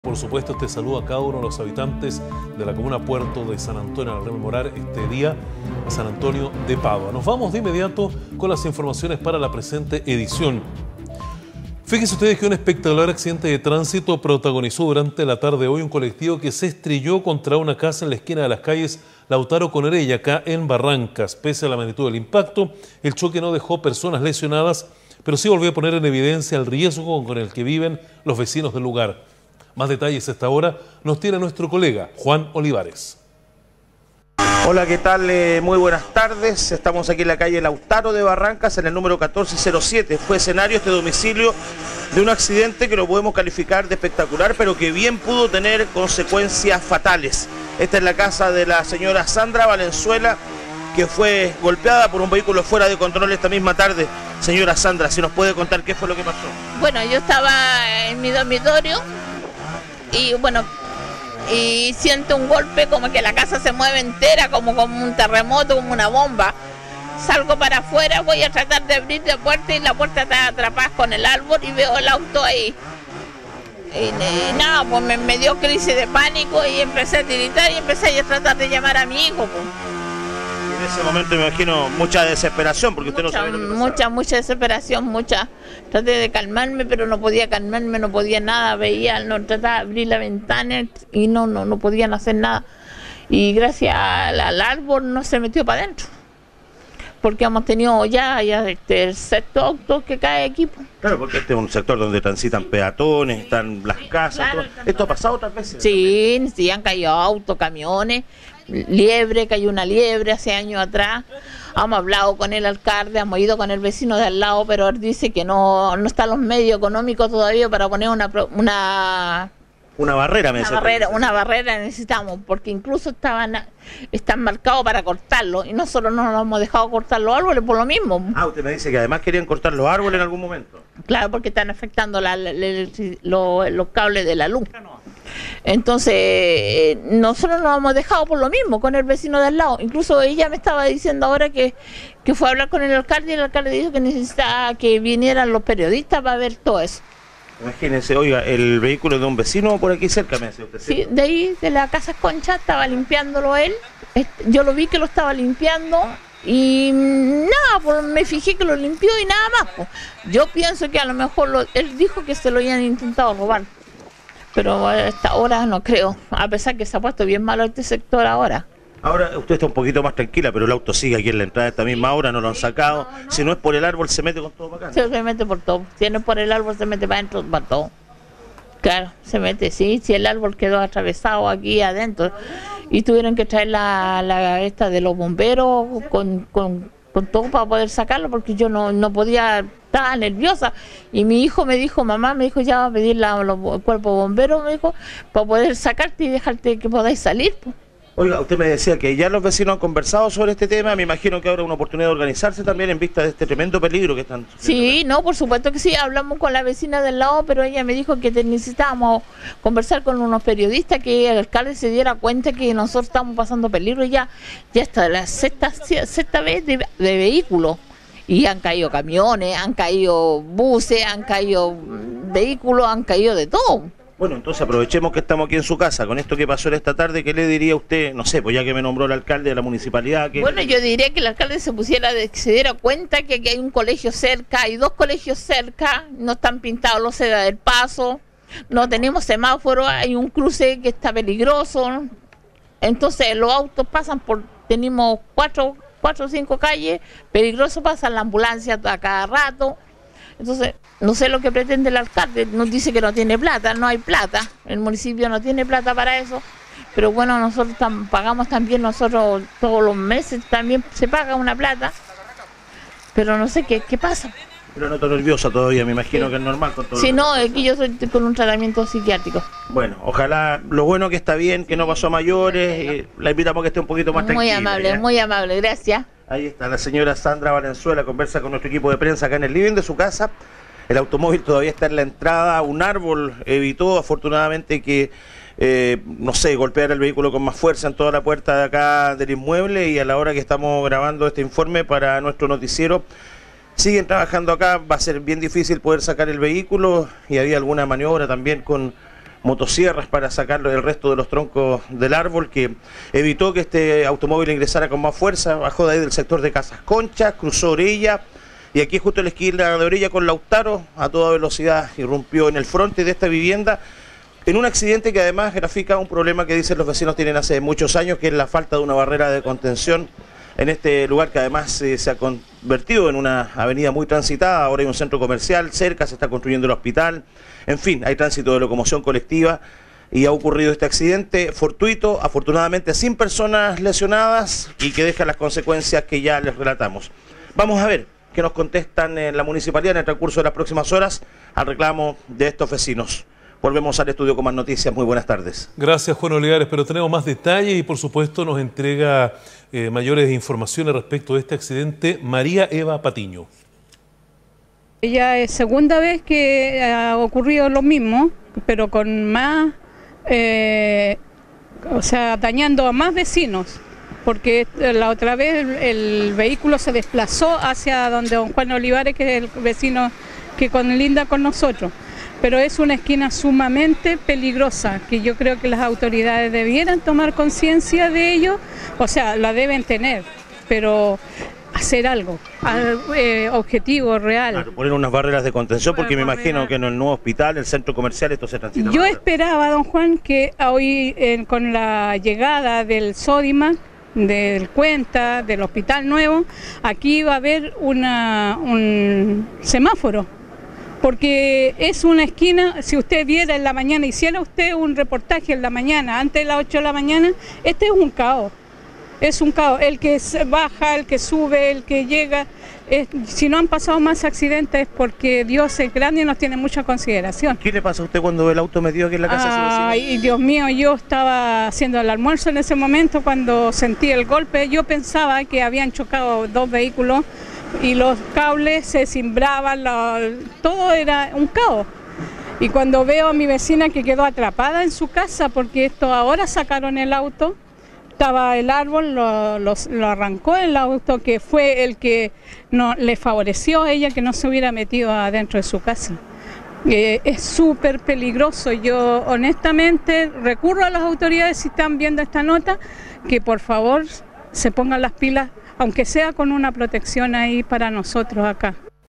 13 JUN 2019 Colectivo choca contra una casa en Barrancas
Un espectacular accidente de tránsito protagonizó un colectivo que se estrelló contra una casa en la esquina de las calles Lautaro con Orella en Barrancas. Pese a la magnitud del impacto el choque no dejó personas lesionadas pero si volvió a poner en evidencia el riesgo con el que viven los vecinos del sector. El periodista Juan Olivares tiene todos los detalles en el siguiente reporte, adelante Juan muy buenas tardes.